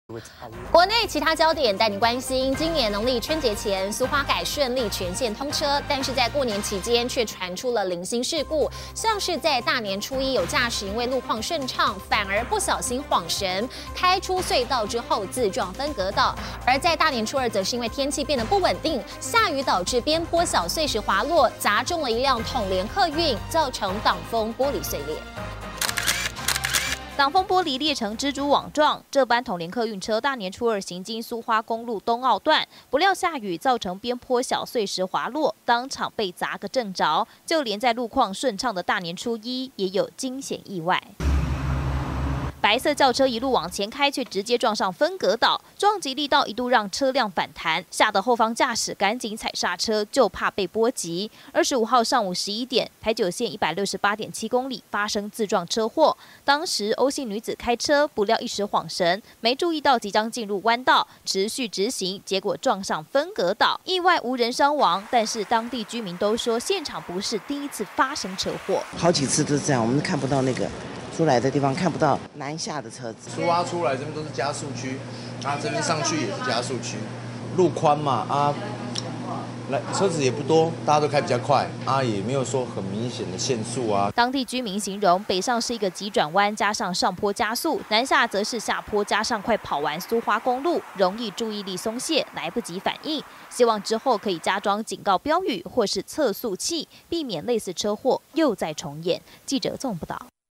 疑一時恍神 花蓮台九線車輛自撞無傷亡
今年農曆春節前蘇花改順利全線通車，不料過年期間卻傳出零星事故。像在大年初一有駕駛因為路況順暢，反而不小心恍神，開出隧道後自撞分隔島，大年初二則是因為天氣變得不穩定，下雨導致邊坡小碎石滑落，砸中一輛統聯客運，造成擋風玻璃碎裂。